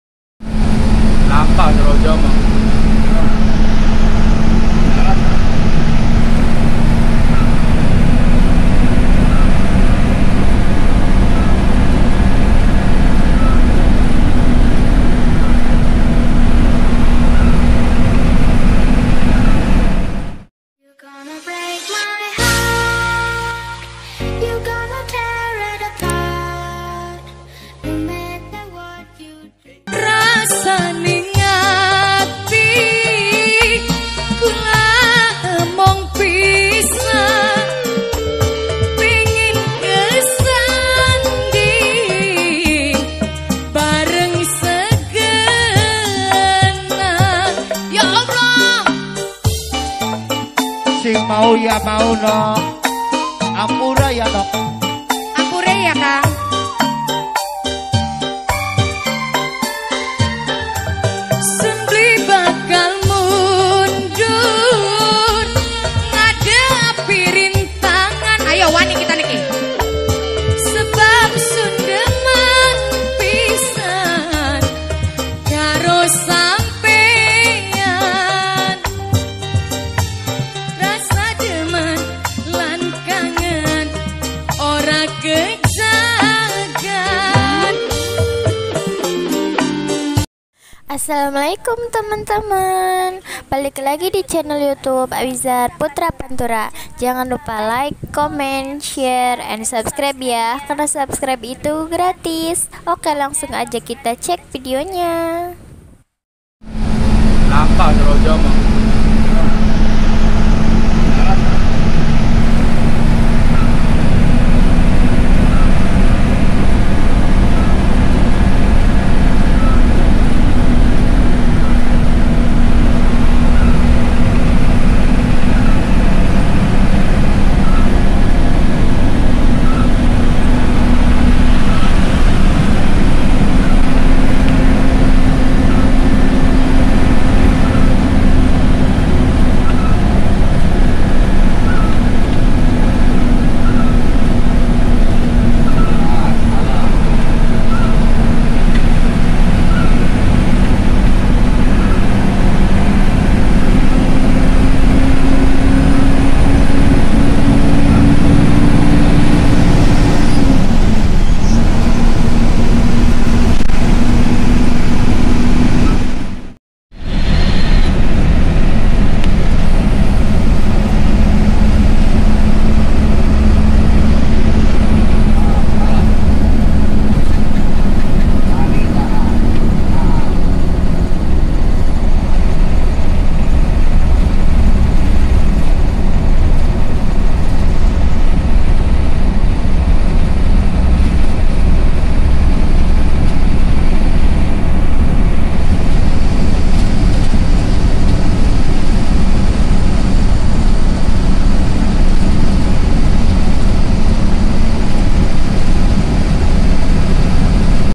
Wow . . Driver Bus Paling Berani Nih🔥
ora mlayu ora payu . .
bus setia negara seroja abatasa ngeblong baju jalan di saat hujan deres nih . .